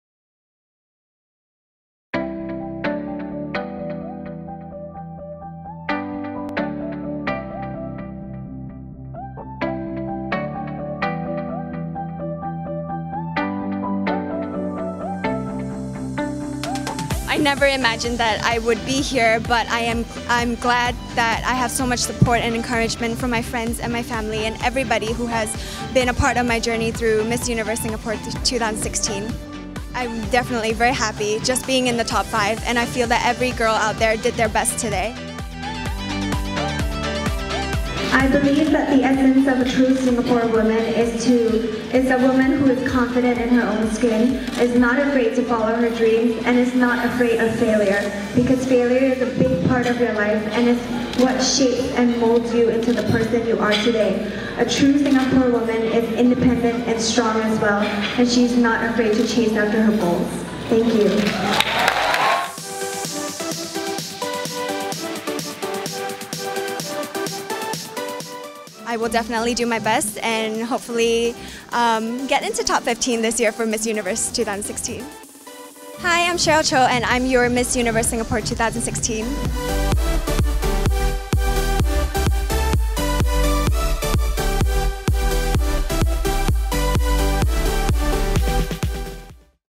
Miss Universe Singapore 2016 is Cheryl Chou
Cheryl Chou wowed the crowd with her answer to the question: What's the essence of a Singaporean woman? She took home the Miss Universe Singapore title at the finals on Sunday, Oct 16.